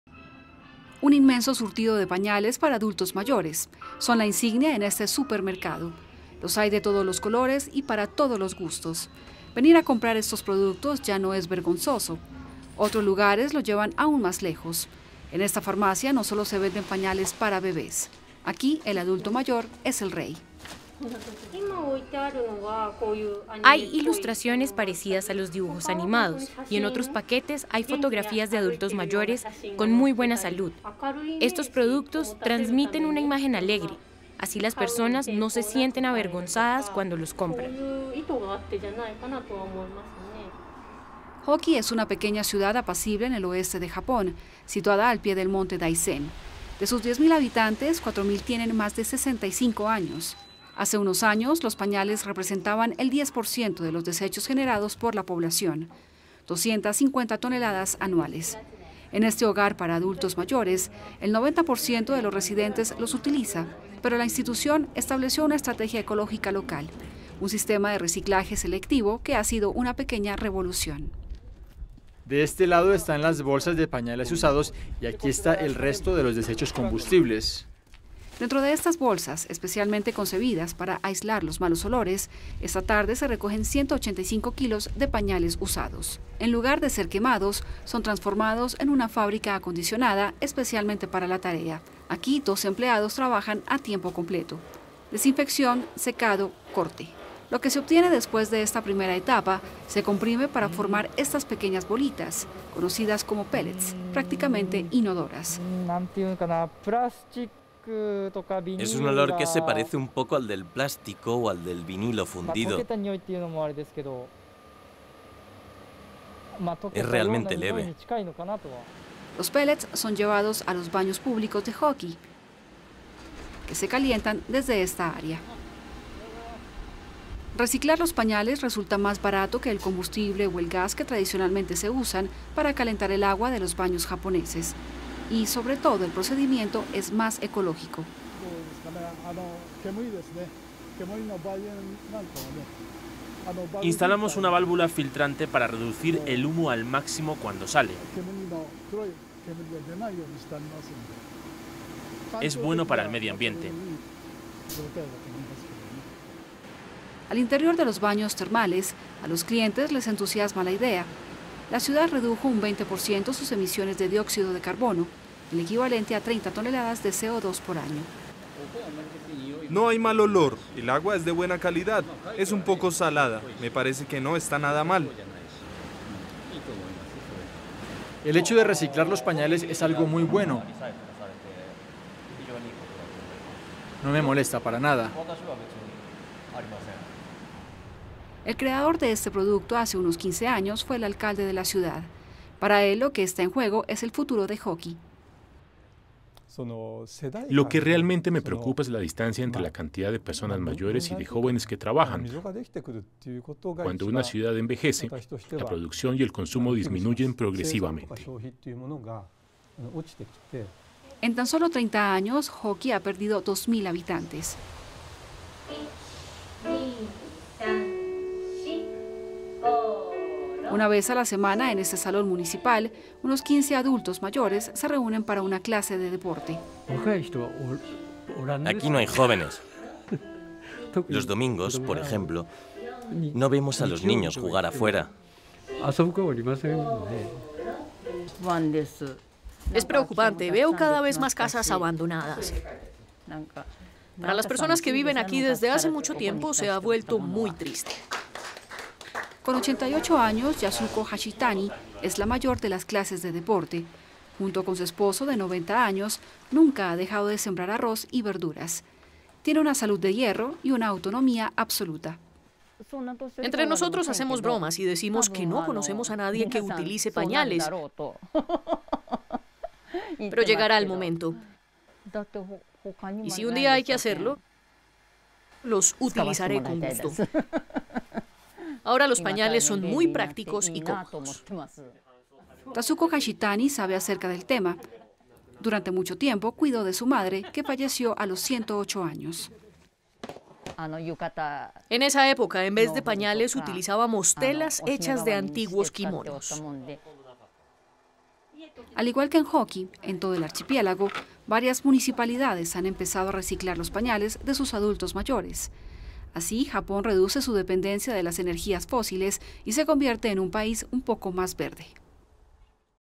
En la ciudad japonesa de Hōki, los pañales para adultos calientan los baños públicos
El envejecimiento de la sociedad de Japón lo está obligando a innovar de formas inusuales. Hace quince años, la ciudad occidental de Hōki, donde casi la mitad de los residentes tienen más de 65 años, comenzó a usar pañales para adultos reciclados para calentar los baños públicos y reducir las facturas de energía. El proyecto fue un gran éxito y ahora la idea de convertir pañales en pastillas de combustible se ha extendido a otras partes de Japón. Informan nuestros corresponsales.